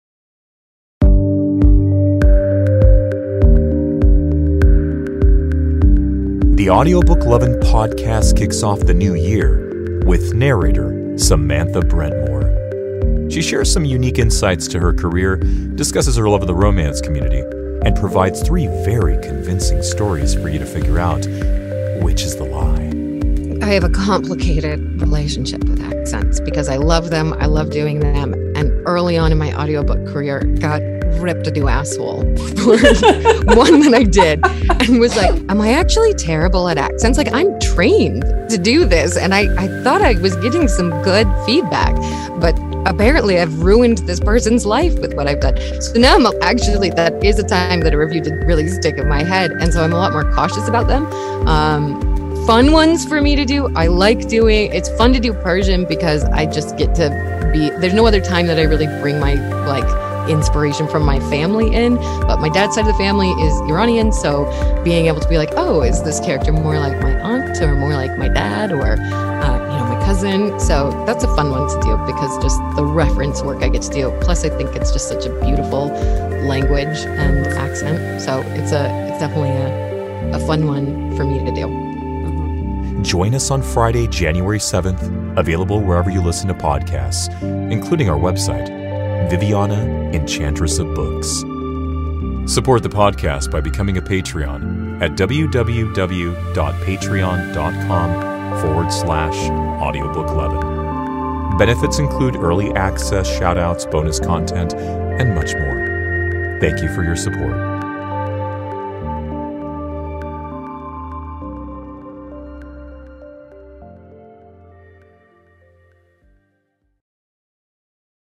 On the next Audiobook Lovin' Podcast - we kick the year off with Samantha Brentmoor
The Audiobook Lovin' Podcast kicks off the new year with Narrator Samantha Brentmoor! She shares some unique insight to her career, discusses her love of the romance community and provides three very convincing stories for you to figure out which is the lie!

Join us on Friday, January 7th, available wherever you listen to podcasts, including our website, Viviana, Enchantress of Books!